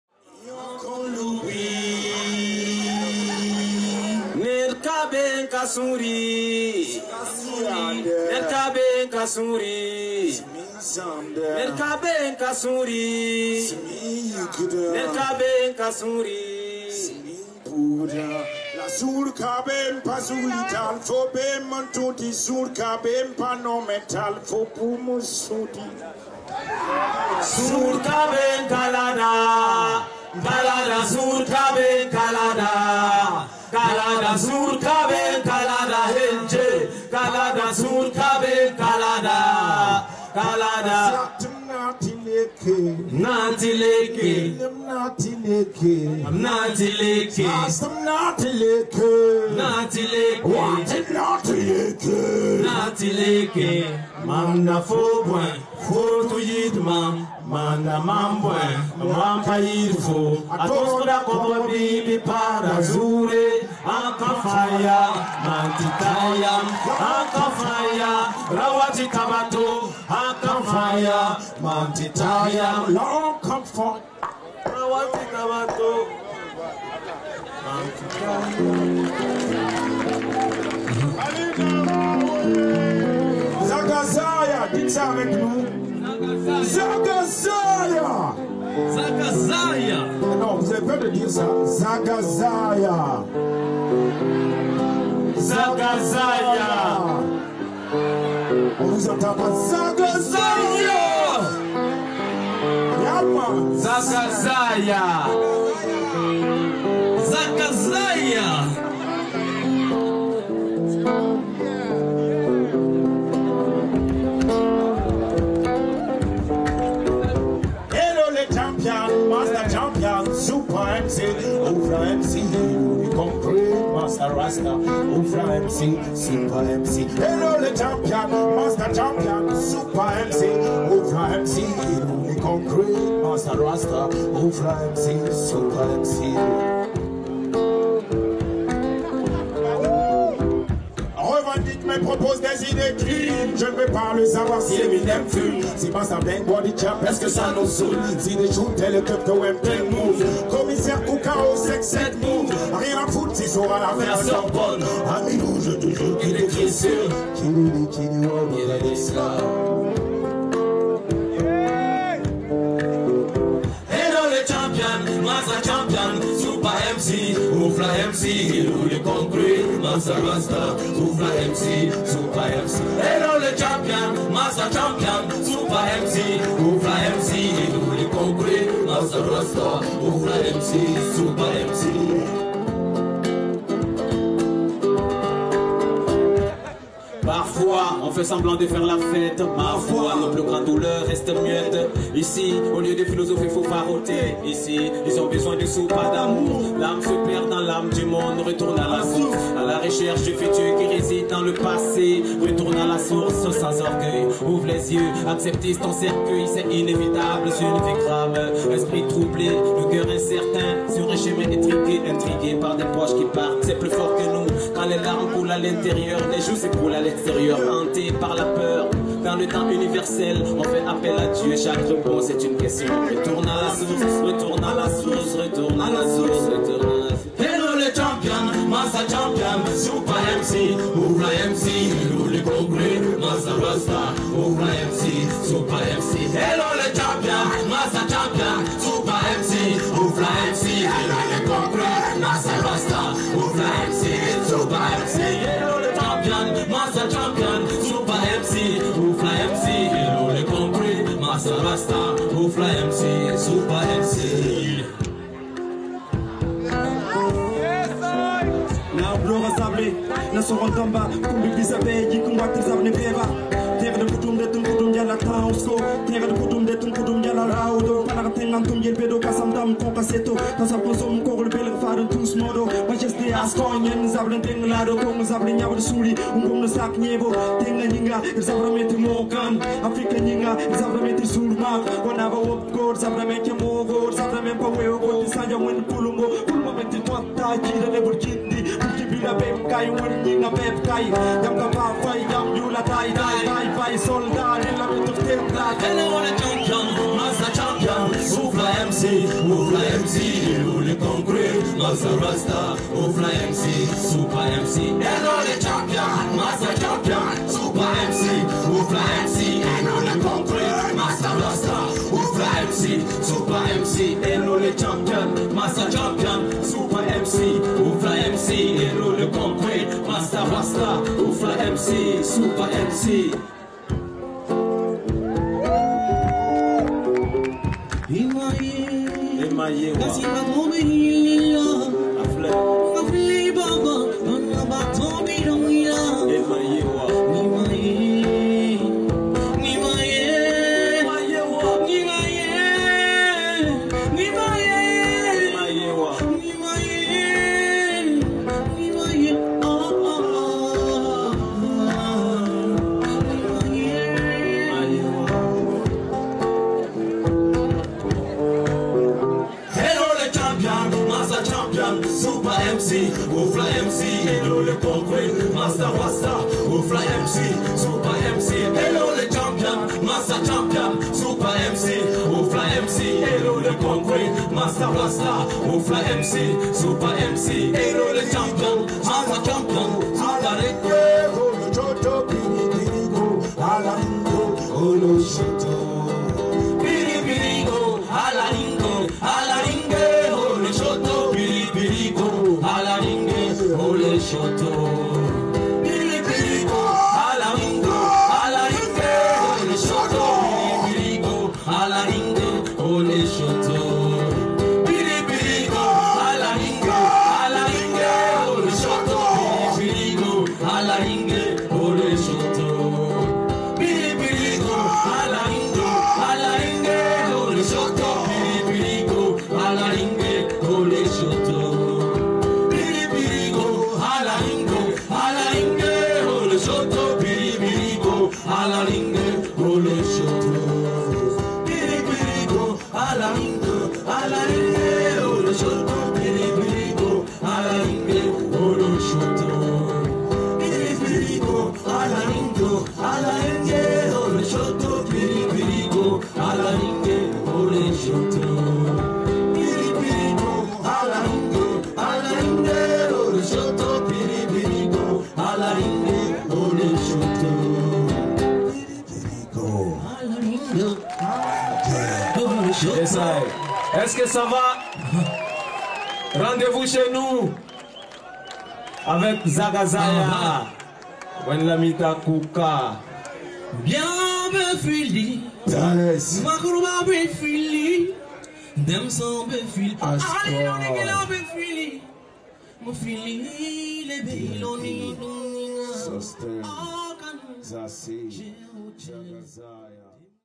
Wendlamita Kouka et les Zaga zaya - LIVE - Festival RDV Chez Nous 2014
Captation lors du Festival RDV Chez nous 2014.